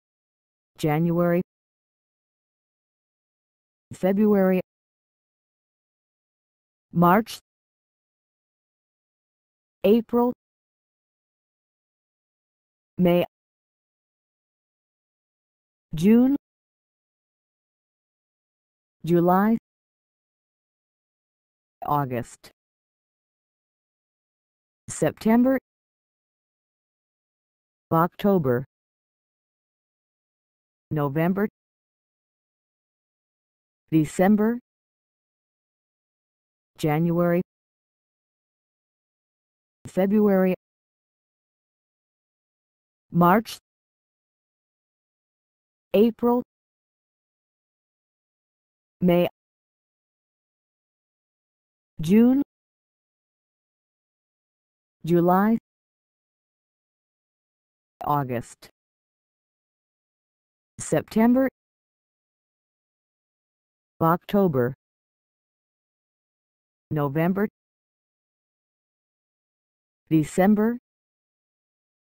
Kids Learning Months of the Year
Learn English Month names- Kids lessons January, February, March, April, May, June, July, August, September, October, November, December